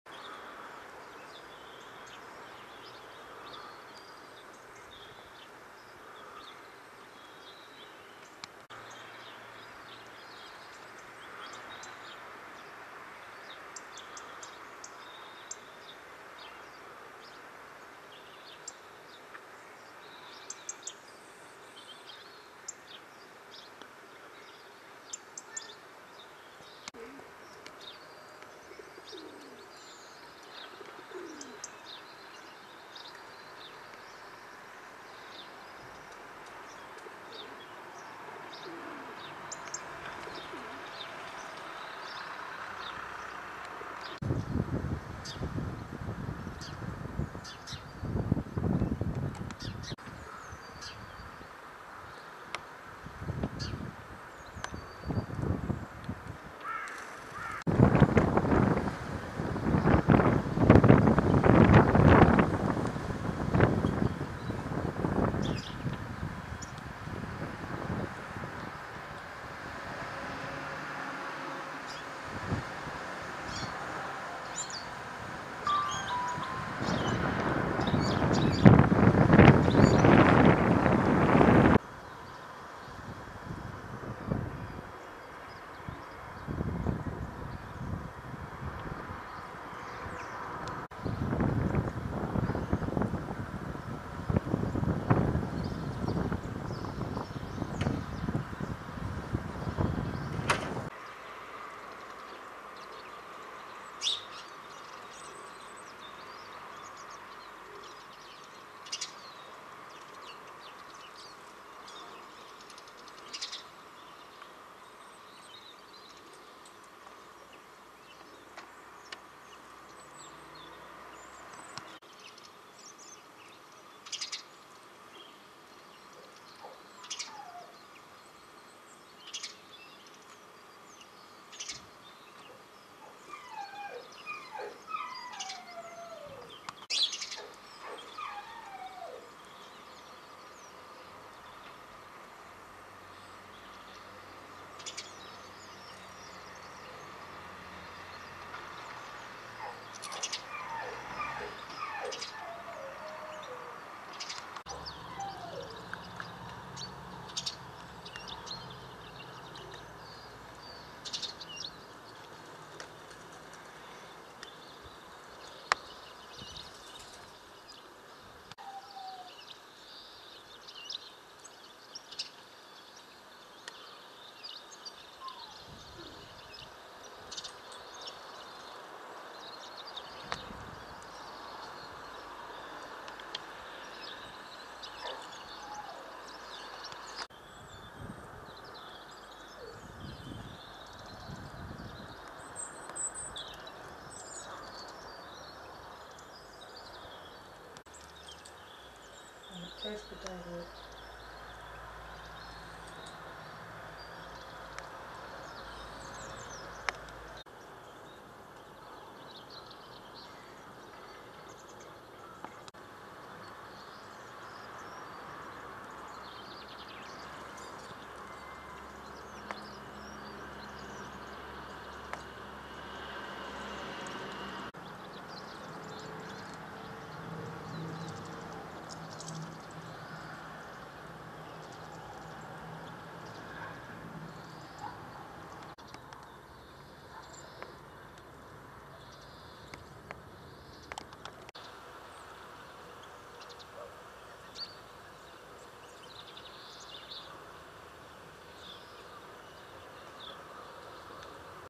Wildlife on Film Volume 6
GARDEN IN KIRN: Siskin, chaffinch, house sparrow, plus no less than five goldfinches feeding.

2010 is kicked off in fine style as siskins make their usual January visit to the garden, as well as the usual favourites.

We also catch a rare glimpse of a couple of house sparrows in the tree.  It is bewildering to think that only just a few years ago the sparrows were probably the most common species in our garden, with over a dozen at a time not unusual.  Sadly their numbers have dramatically declined since and they now appear only occasionally.

But while the house sparrows have been on the decline, goldfinches on the other hand have been on the up, with several seen on this particular video.

Camera used: Panasonic SDR-S26